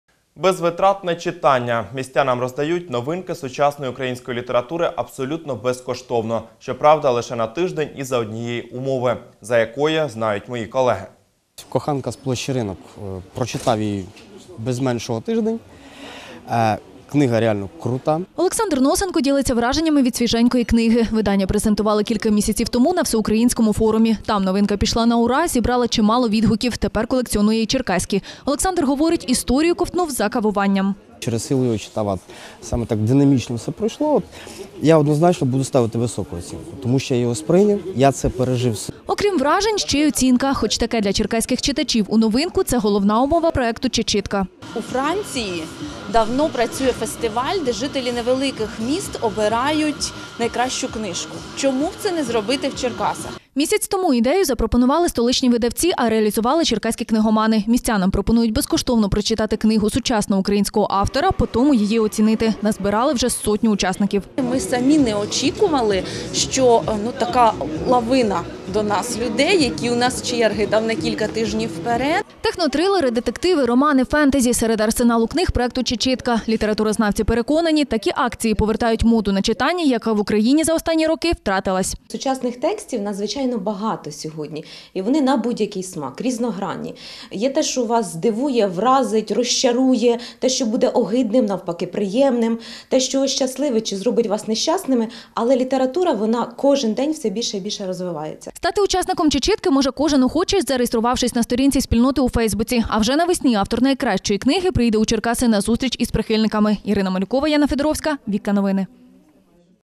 Черкаські книгомани визначають найкраще вітчизняне видання року
Містянам роздають новинки сучасної української літератури абсолютно безкоштовно